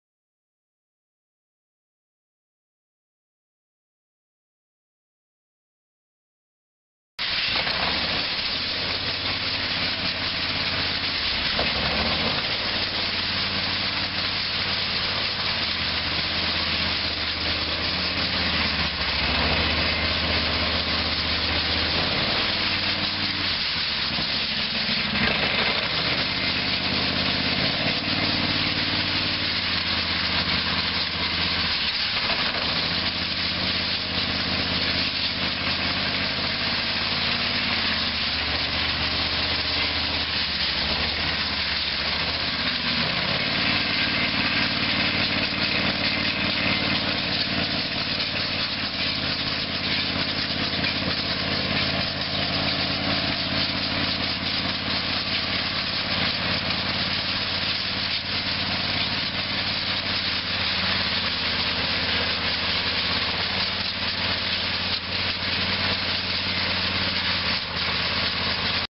2002 Razorback Rider Pro 1200 Professional Series ride-on power trowel | auction September 25, 2012
SOLD! Bobcat of the Rockies Auction

2002 Razorback Rider Pro 1200 Professional Series ride-on power trowel
787 hours on meter
Kawasaki FD750D gas engine
27 HP
Vandel guard control
Cure applicator sprayer
(2) trowels
Lights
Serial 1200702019
Unit# 449671

It was located in Parker, CO, and we sold it to the highest bidder for $1,760.